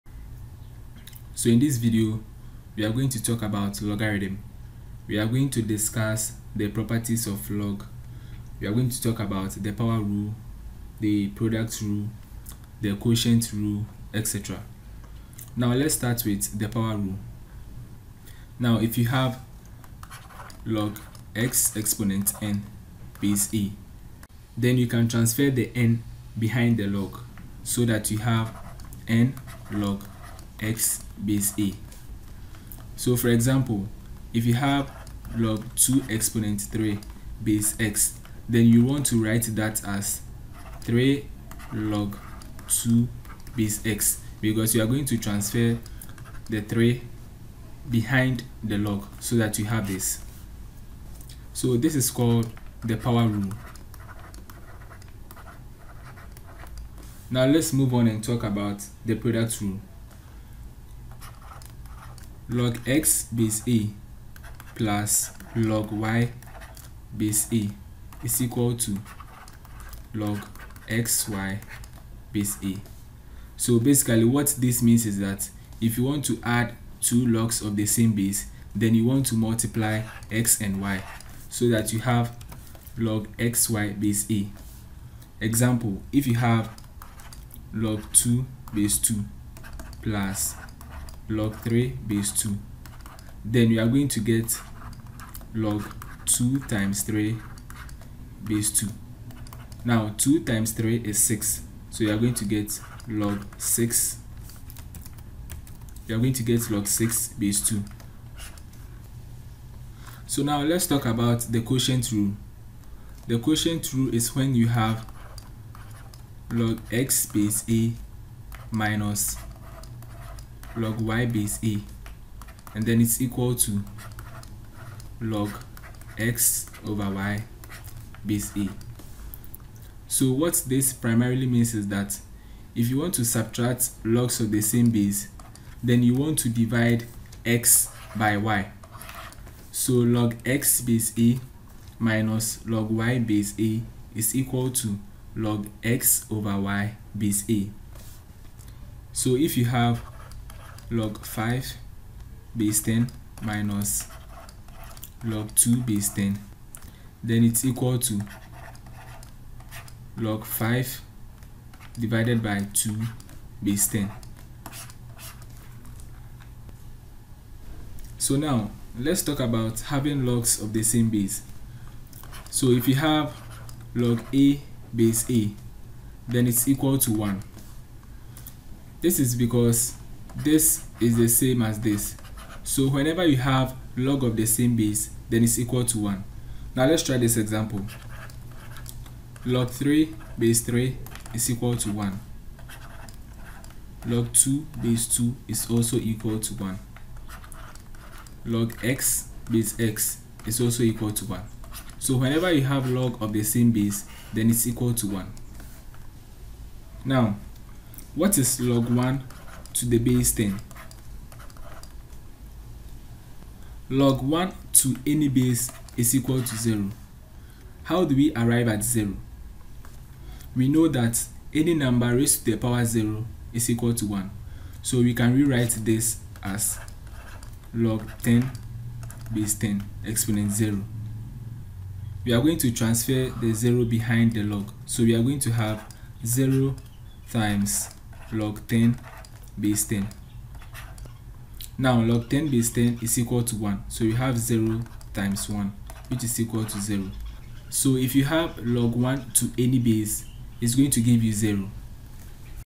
Introduction to Logarithms | SHS 2 CORE MATH
In this video, the properties of log are explained. the power rule, product rule, quotient rule, etc was discussed. Log of the same base is equal to 1.

Ghana
For all Ghanaian Senior High School Students
Elective Maths and Core Maths
WASSCE CORE and ELECTIVE MATHS 2020